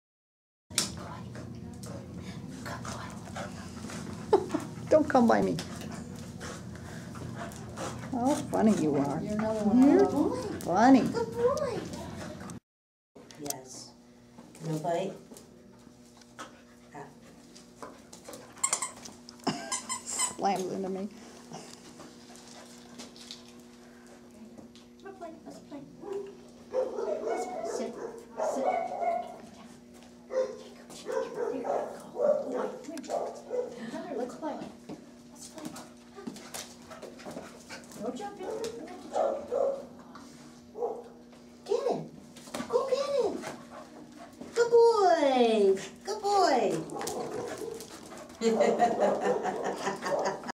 Meet Travis a Australian Cattle Dog currently available for adoption at Petango.com! 2/18/2013 9:45:
Travis is a rambunctious, playful, energetic little pup looking for the perfect owner who will have patience with him and work with him. He is a smart dog, so he will catch on to training quickly. Travis will need regular exercise and he would love a big backyard to play in. Travis is only 8 months old, so he is still a pup!